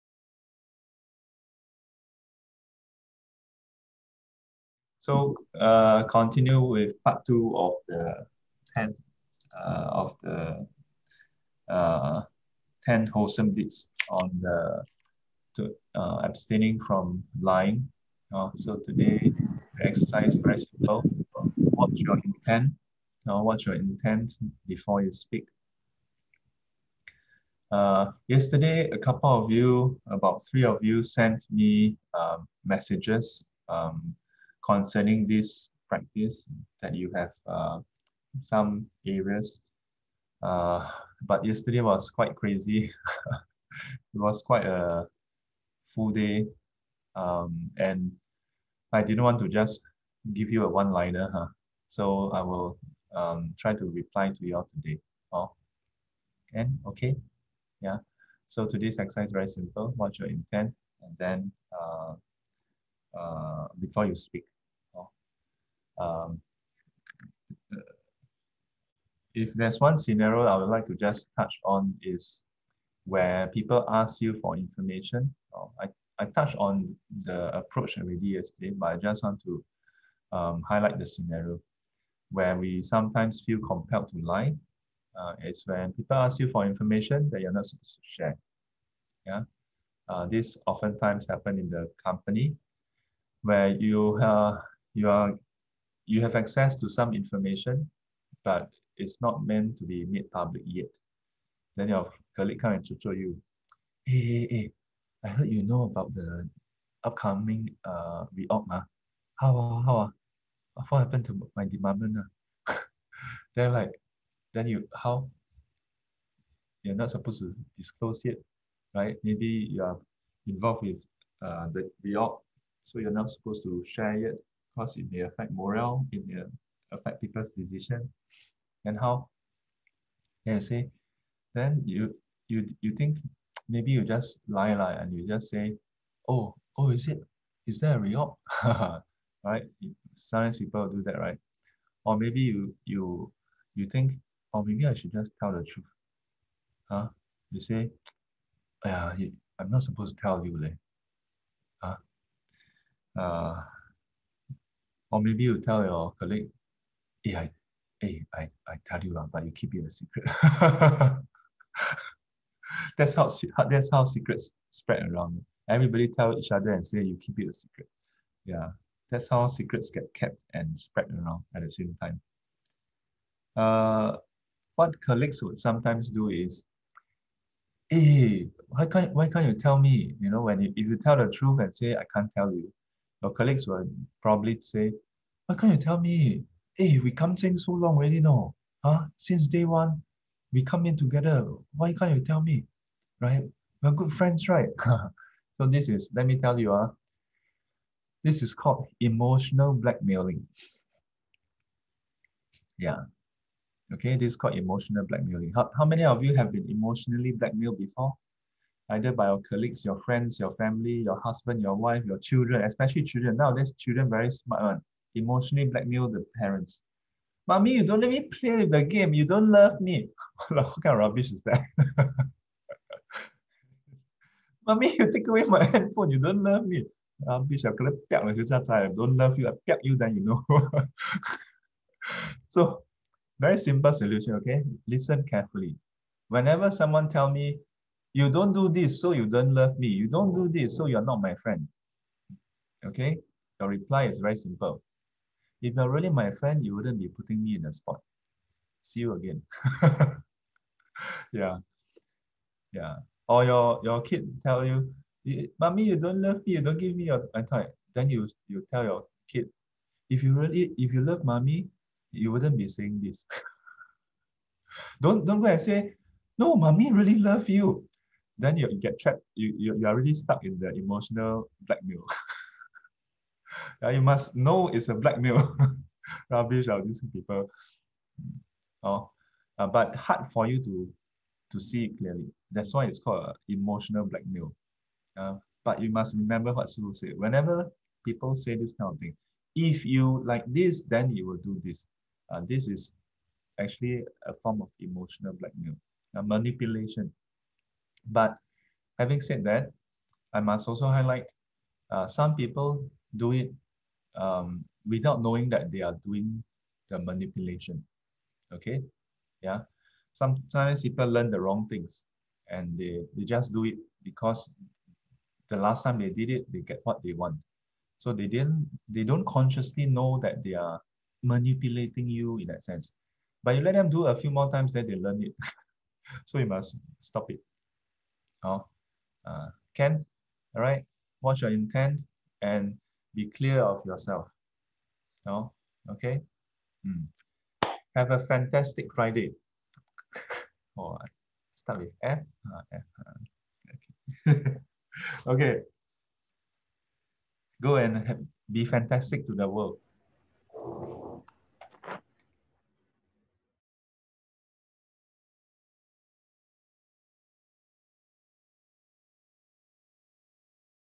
4th Wholesome Deed: Not Lying (Part 2) | Emotional Blackmail
The second exercise of the fourth wholesome deed, not lying.
What is emotional blackmail?

Find out now in our Daily Dose of Dharma!